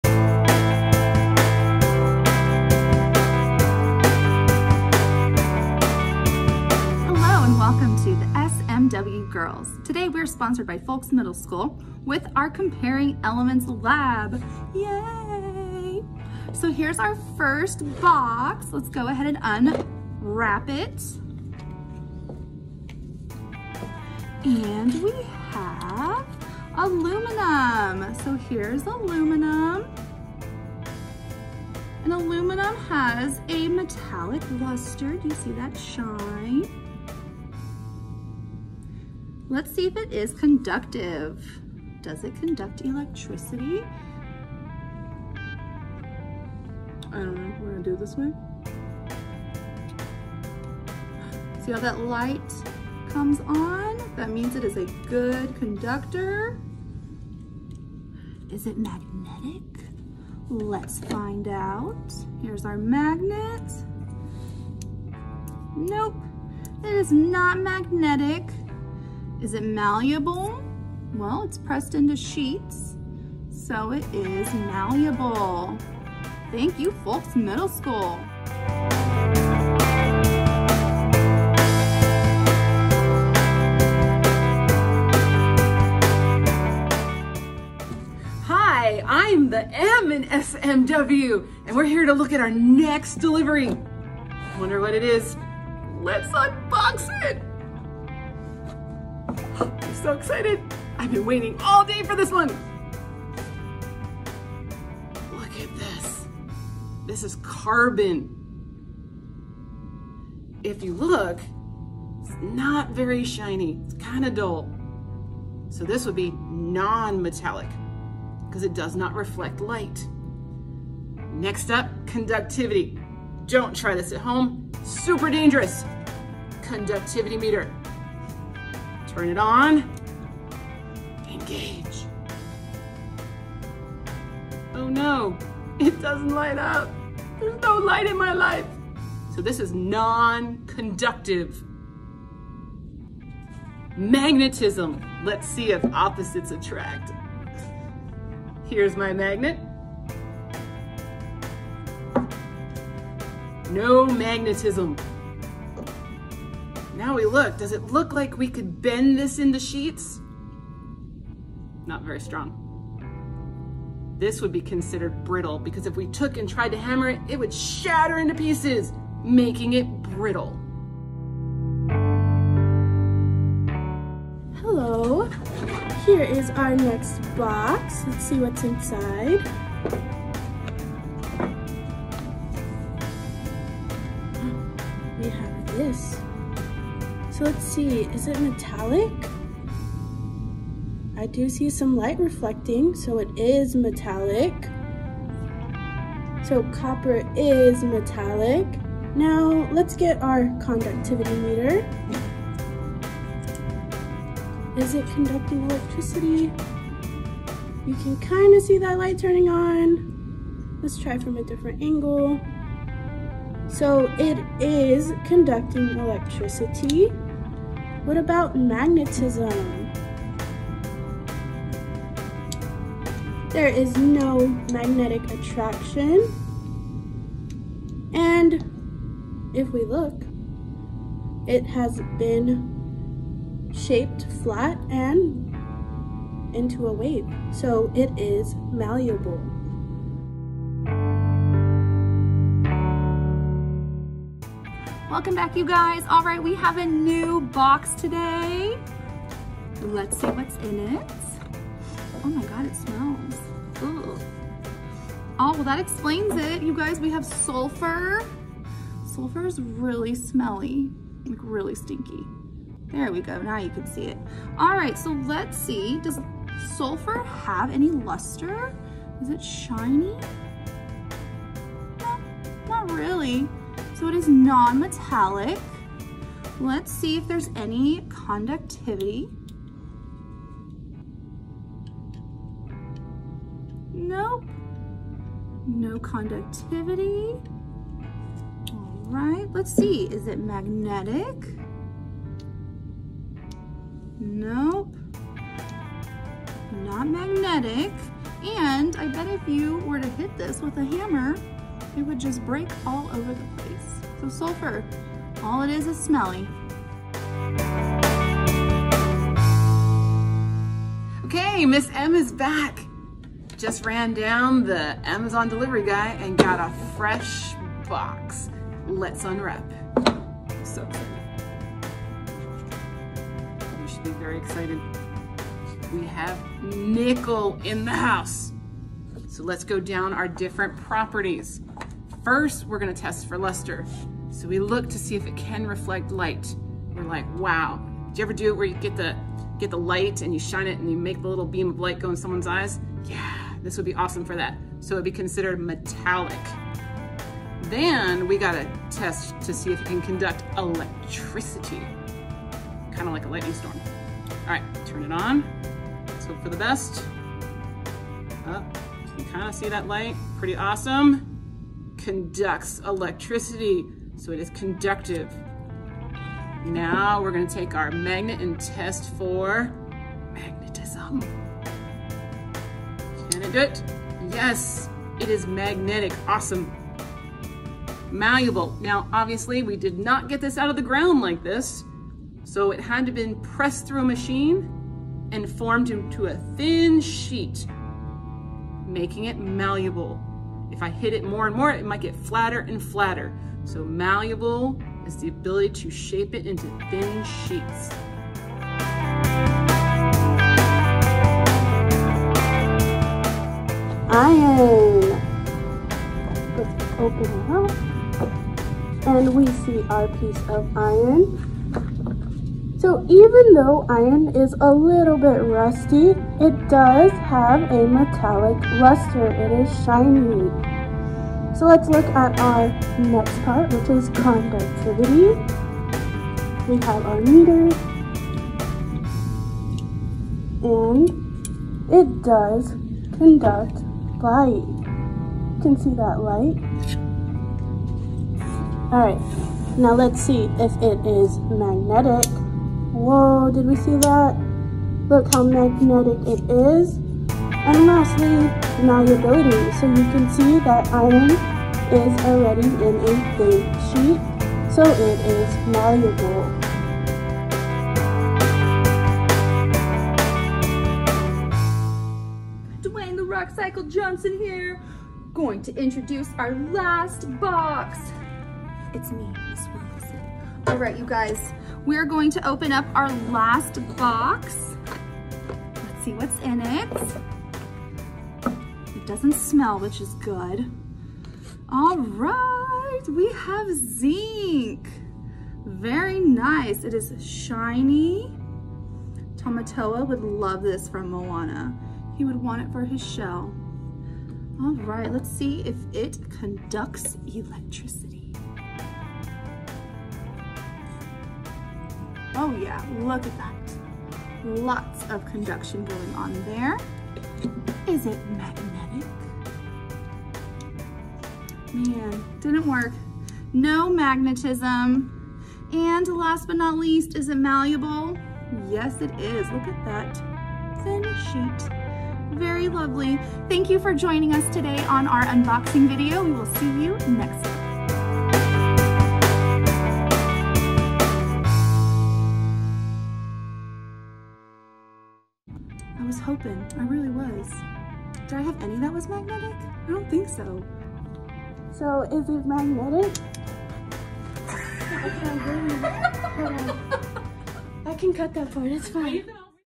Unboxing Elements
Join the SMW Girls as we unbox some new elements and describe their physical properties. Will they be shiny? Conduct electricity? Will be they malleable? Do opposites really attract? Find out in our video.

*no scientists were harmed in the making of this video*